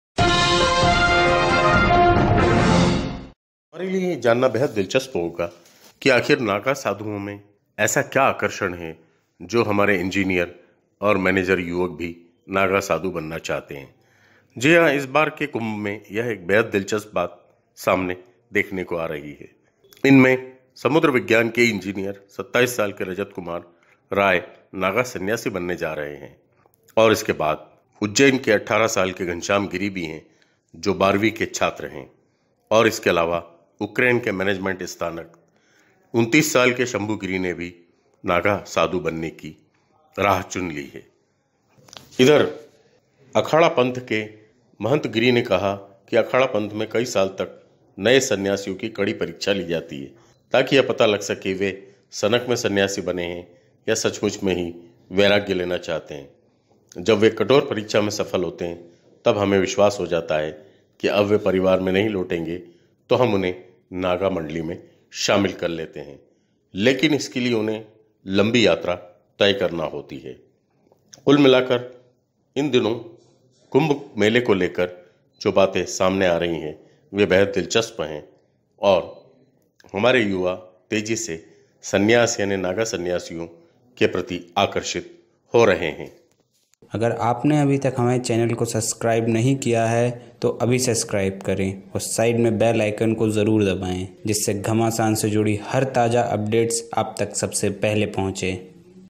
Watch 'Naga Sadhu' - Engineer युवक आखिर क्यों बनना चाहते हैं 'Naga Sadhu': Kumbh का रहस्य
Naga Sadhu  का ऐसा क्या है रहस्य